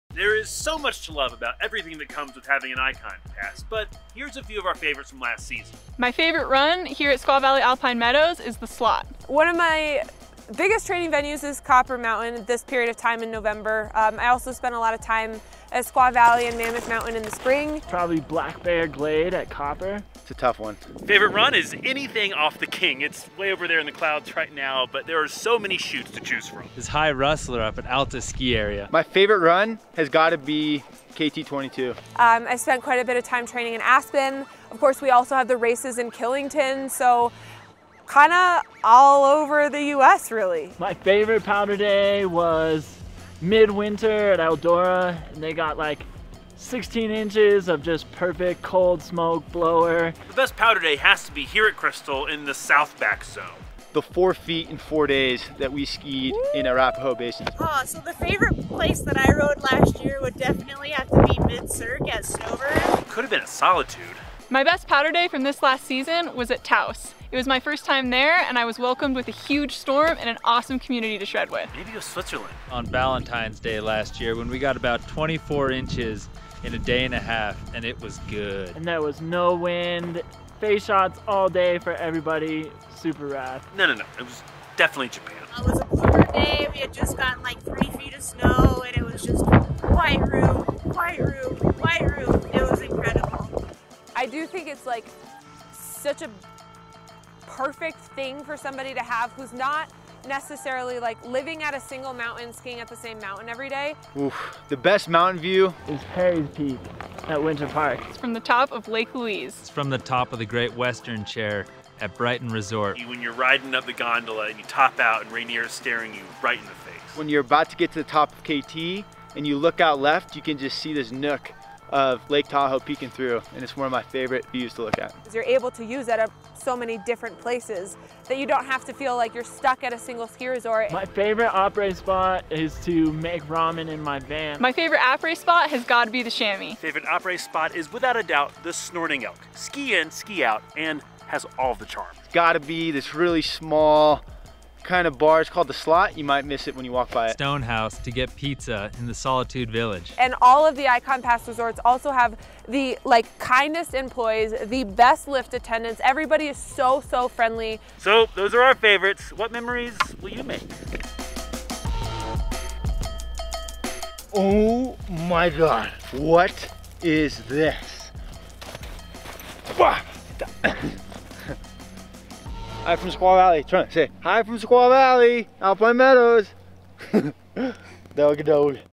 A Few of our Favorite Mountain Moments
To our IkonPass Holders new and returning, Happy InternationalMountainDay and welcome to our community. These are a few of our pass holders' favorite moments from winter 19/20, and we can't wait to start forming new memories with you for winter 20/21.

Here's to powder days, ramen in the parking lot, chairlift laughs, back bowl turns, and a community of people who feel more at home on snow.